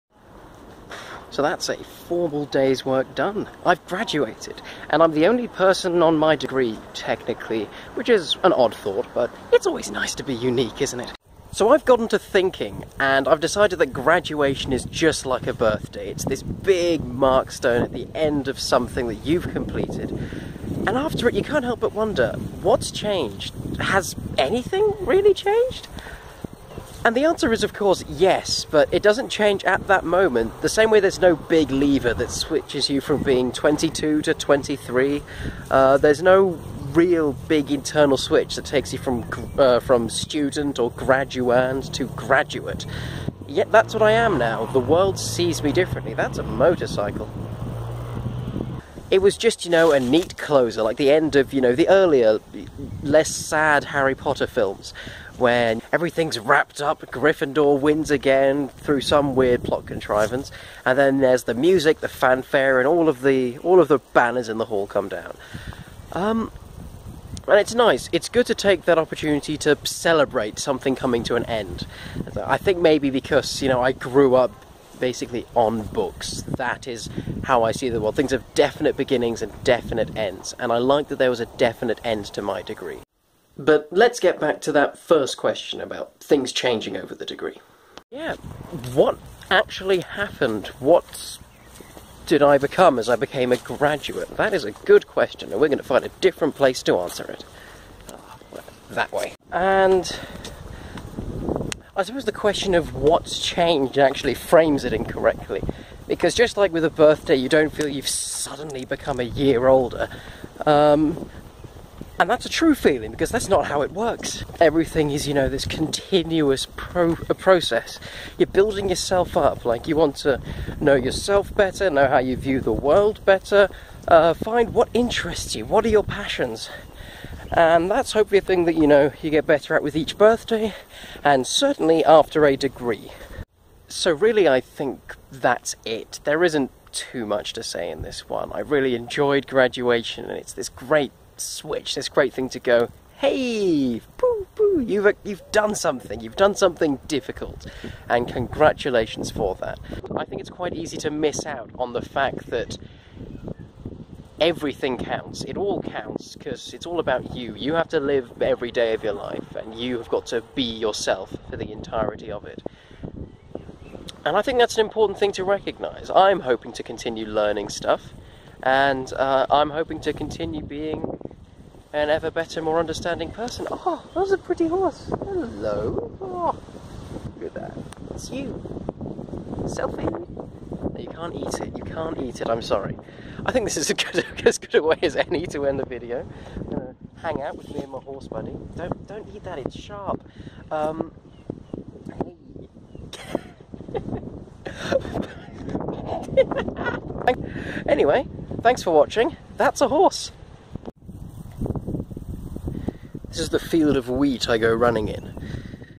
Graduating is like a Birthday (ft. wind noises)
Just some thoughts on graduating and how it compares to birthdays - that feeling of marking a big change that you just can't feel. Also there's a horse and lots of wind sounds.

I'll hopefuly get better at vlogging soon, but bear with me for now!